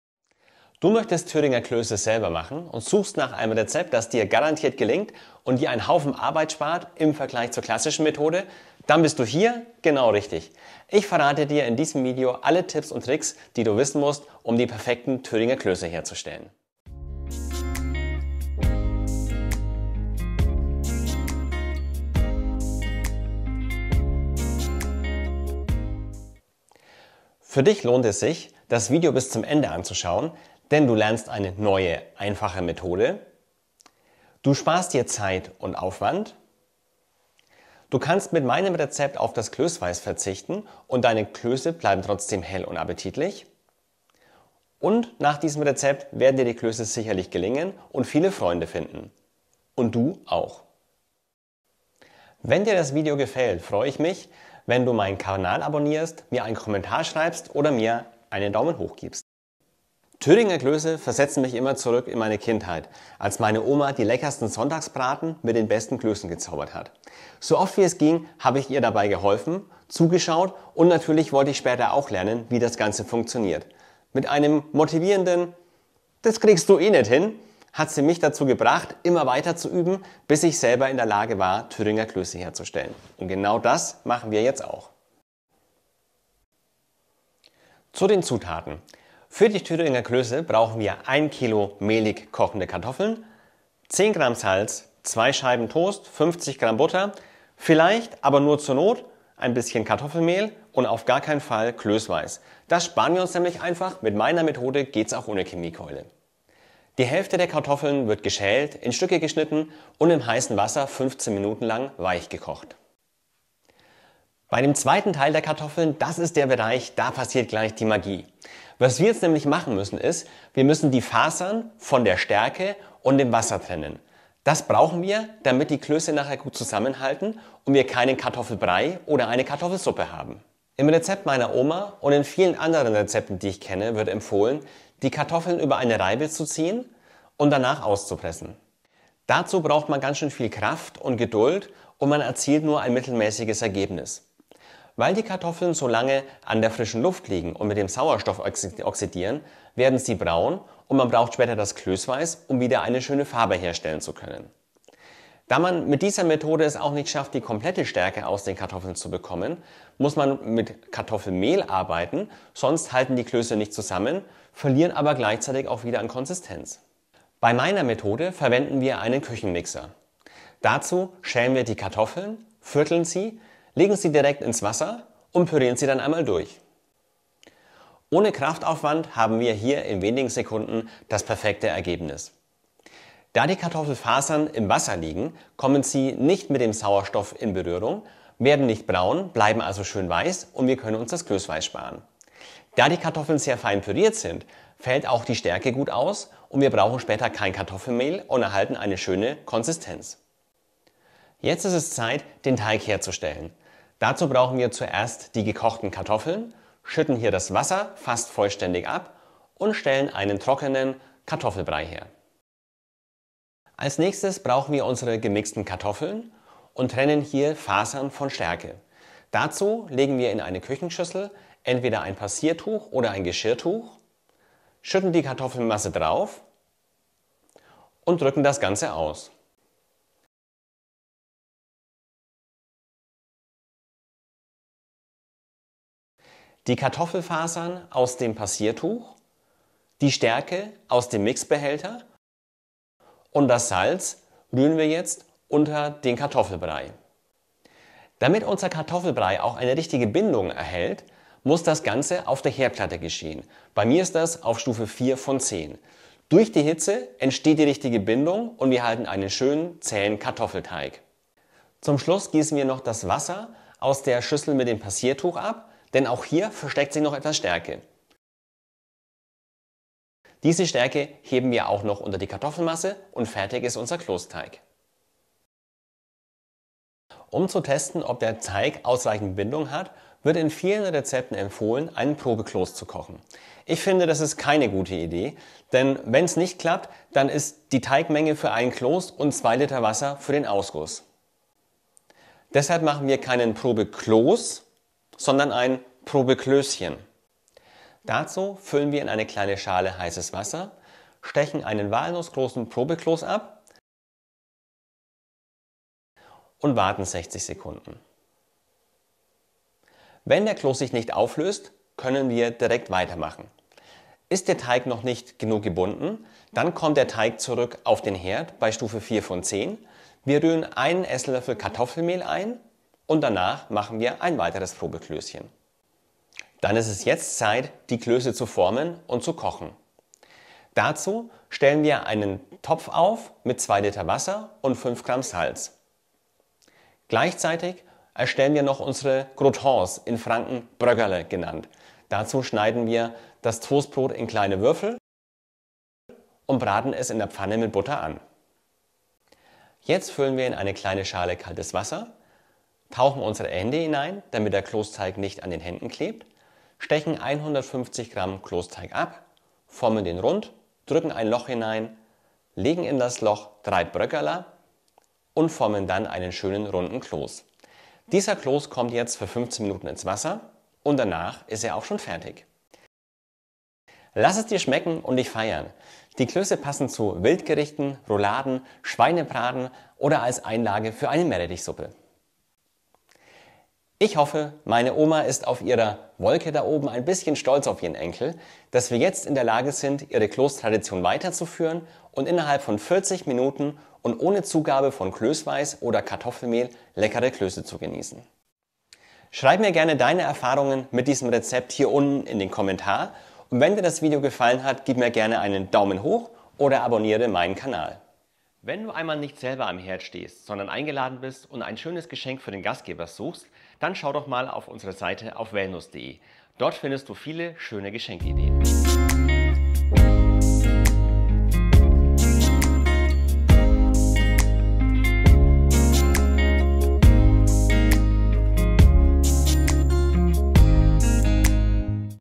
Thüringer Klöße - einfach wie nie - gelingen garantiert in 40 Minuten (inkl. Kochzeit!) (2021)
Thüringer Klöße versetzen mich immer zurück in meine Kindheit, als meine Oma die leckersten Sonntagsbraten und die besten Klöße gezaubert hat. Ich habe meiner Oma beim Klöße machen so oft wie möglich geholfen und wollte von Ihr lernen, wie das funktioniert. Mit einem gut gemeinten "Das bekommst Du nie hin" hat sie mich motiviert, so lange zu probieren, bis ich es selbst kann und jetzt das Rezept, mit dem die Klöße einfach gelingen, weitergeben kann.

Das vollständige Rezept für die Thüringer Klöße findest Du

Ein besonderes Dankeschön sage ich meiner lieben Schwägerin und meinem lieben Schwager, die mir zu Weihnachten eine neue Kochjacke mit eingesticktem wellnuss Logo geschenkt haben. DANKE!

Und auch meiner Schwester sage ich vielen Dank: ursprünglich habe ich anstelle eines Küchenmixers einen Entsafter verwendet, um für die Thüringer Klöße Stärke und Fasern der Kartoffeln zu trennen. Meine Schwester hat aber keinen Entsafter und angemerkt, dass mehr Menschen einen Mixer haben, als einen Entsafter. Dadurch bin ich auf die Idee gekommen, mit einem Mixer zu arbeiten und nach ein paar Testversuchen habe ich erkannt, dass ein Mixer noch viel besser funktioniert, als ein Entsafter.

Jetzt wünsche ich Dir viel Spaß beim Ausprobieren und bin gespannt auf Dein Feedback zu diesem Rezept für Thüringer Klöße.